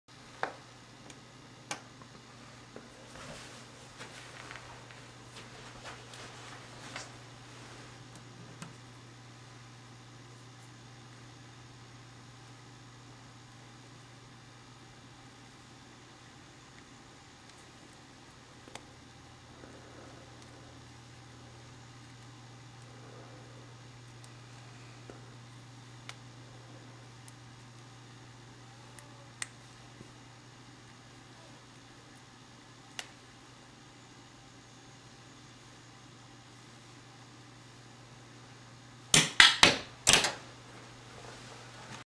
How to Make a Paperclip Fly.mpg
In this video you will learn how to make a common paperclip fly.  It's great for entertaining yourself or others with a common office item.  City Communications specializes in providing completely managed telecommunications services to businesses across the country.  Telecom is our life.  Making paperclips fly is just a hobby.